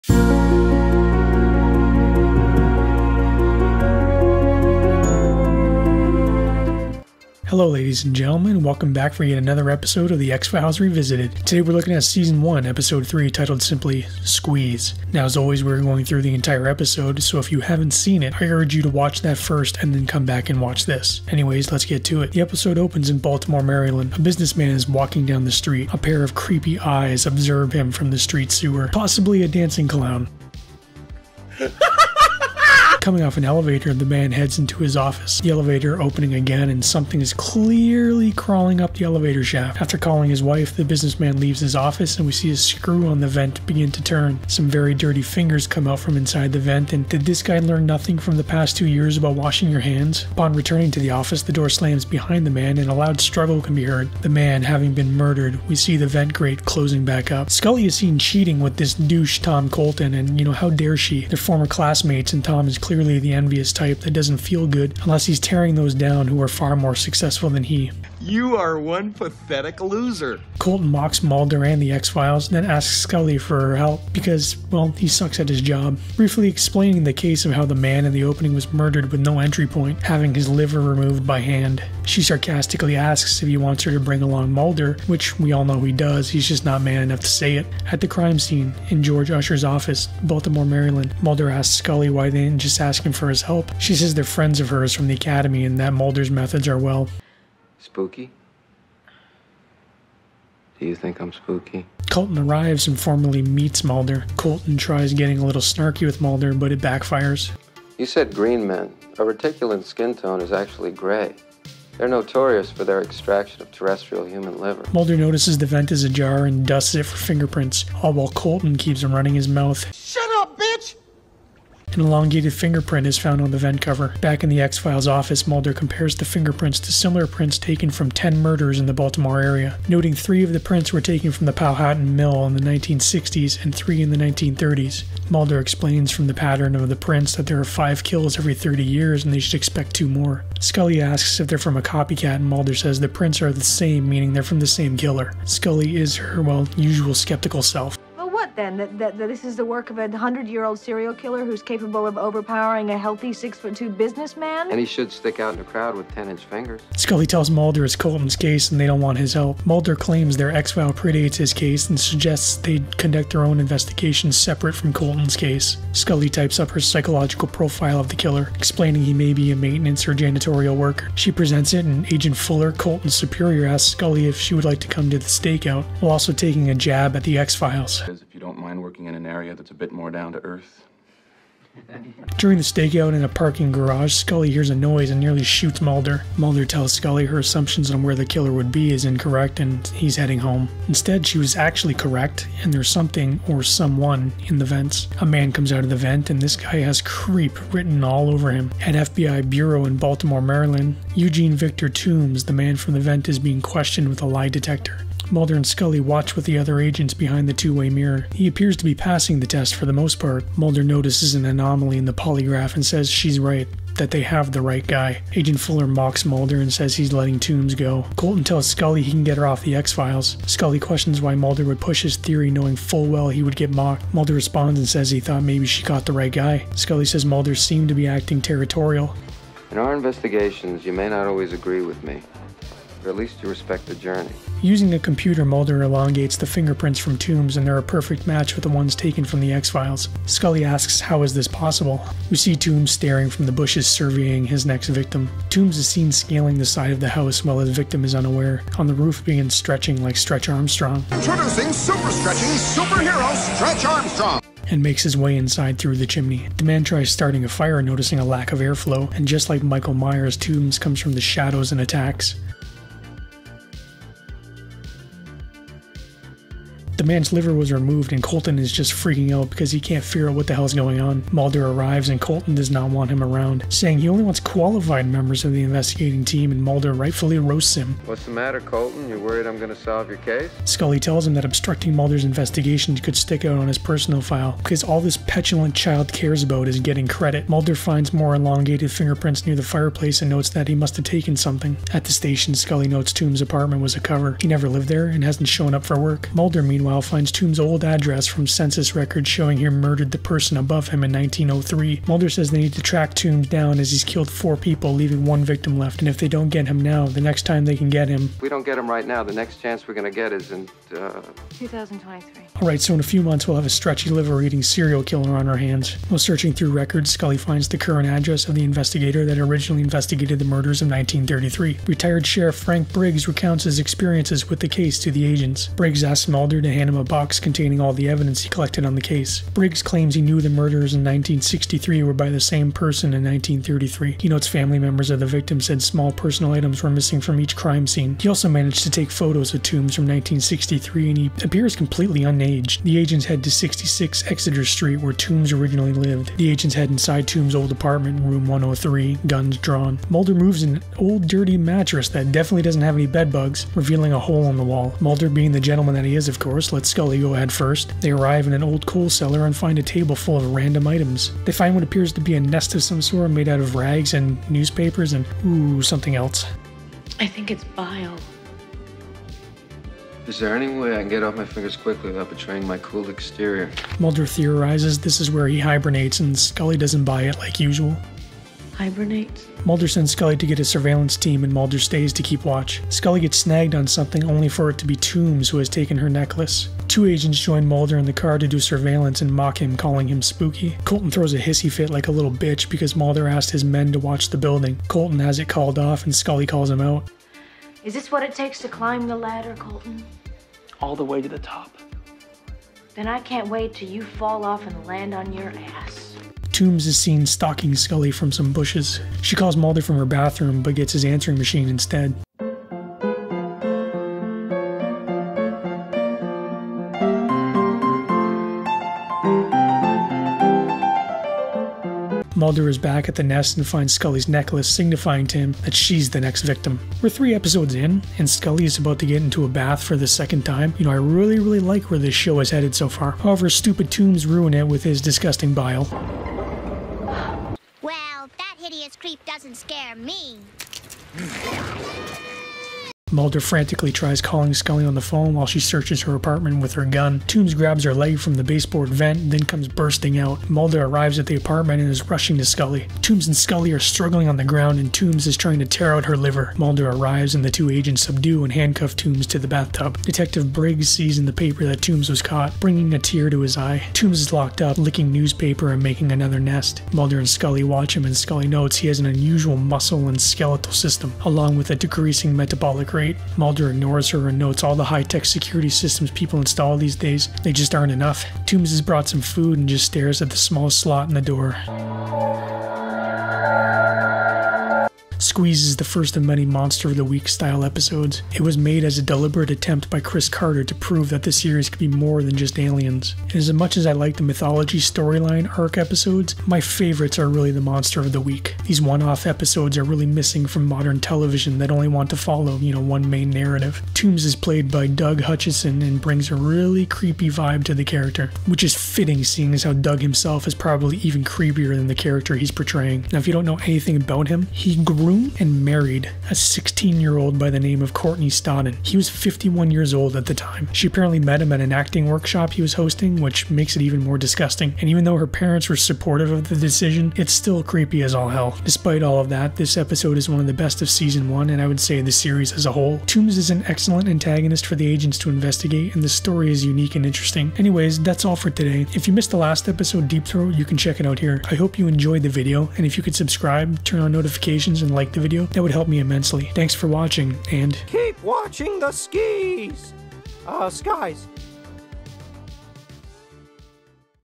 Squeeze S1E3 - The X-Files Revisited
Let's dive into the very first monster of the week episodes.